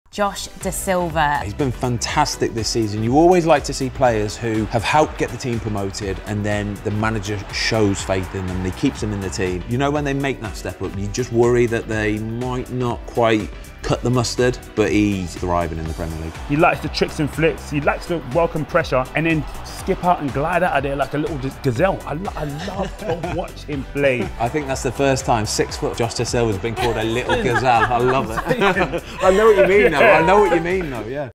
“He glides like a little gazelle!”
“He glides like a little gazelle!” Ben Foster and David Alorka analyse Brentford midfielder Josh Dasilva.